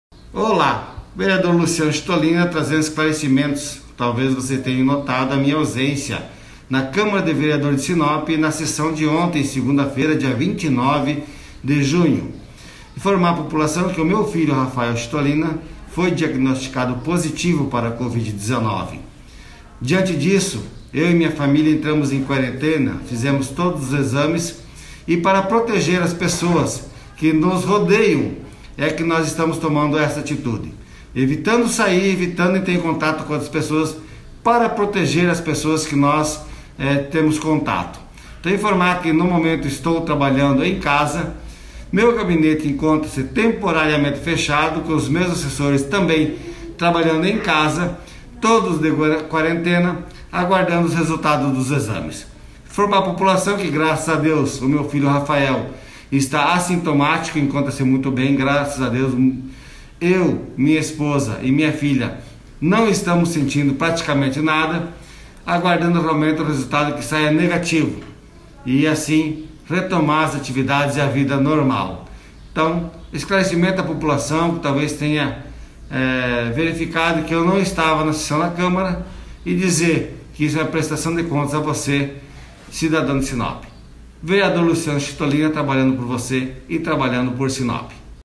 Filho de vereador está com coronavírus e parlamentar fica em isolamento
Vereador Luciano Chitolina está em Isolamento Domiciliar por medida de precaução e pensando no bem da coletividade. Da mesma forma os assessores do gabinete encontram-se temporariamente atendendo apenas virtualmente e pelo telefone.